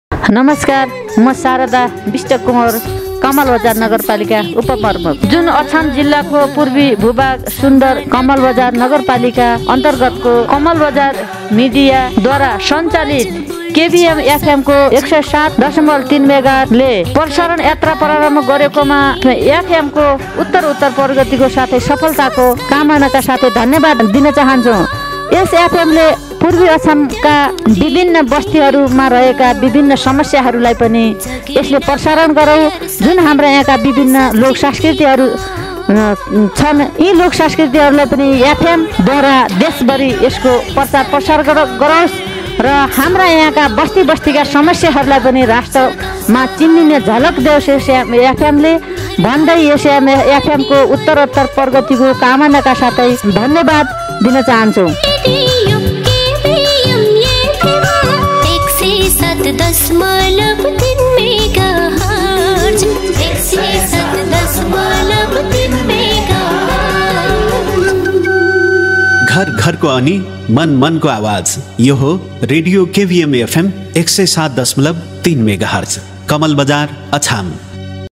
कमलबजार नगर उपप्रमुख शारदा कुमारी बिष्ट कुँवर द्धारा KBM FM 107.3 MHz लाइ प्रगतीकाे शुभकामना व्यक्त
KBM TV Kamalbajar Media Pvt Ltd Kamalbazar Media
Hikmat Nepali KBM TV  Kamalbazar Update Achham Update Khabar